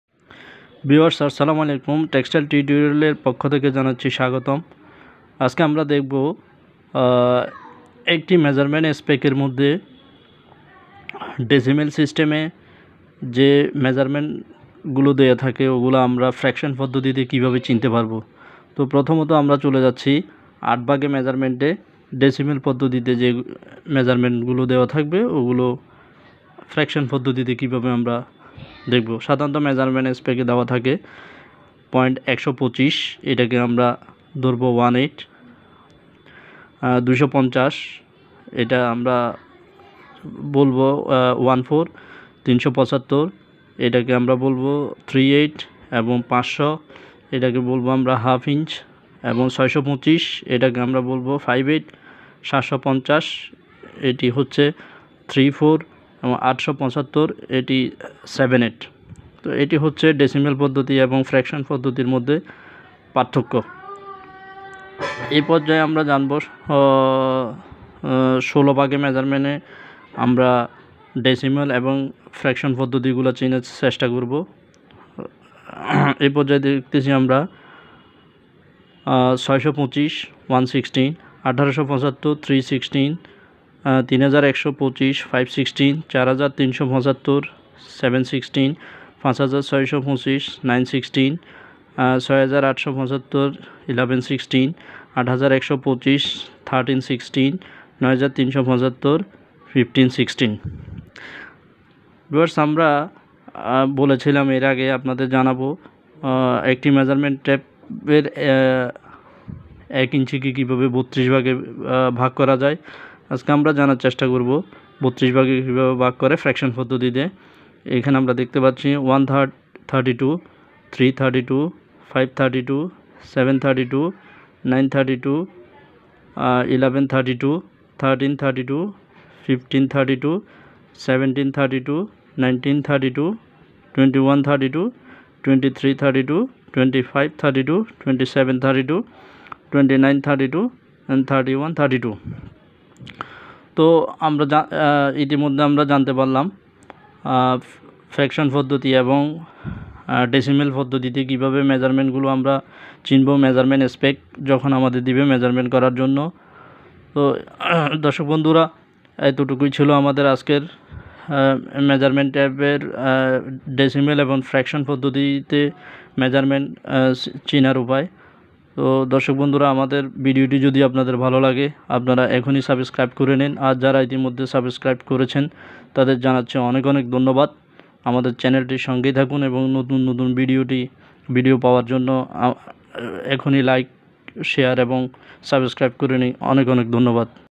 introduction to measurement spec.
In this video i will show fraction and decimal measurement digit in measurement spec. Anyone can learn relation between fraction and decimal im spec.